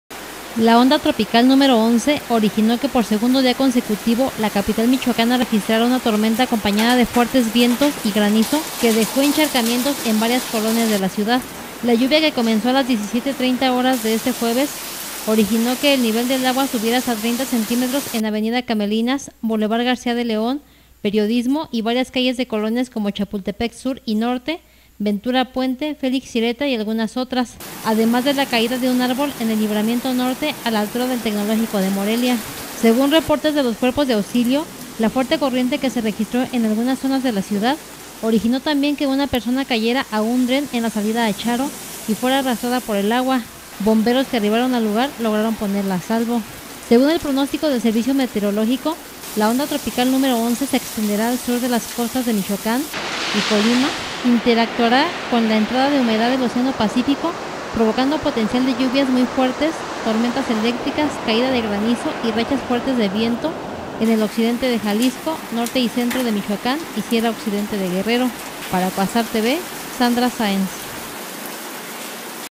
Tormenta origina encharcamientos y caída de un árbol en Morelia
La onda tropical número once originó que por segundo día consecutivo la capital michoacana registrara una tormenta acompañada de fuertes vientos y granizo, lo que originó encharcamientos en varias colonias de la ciudad…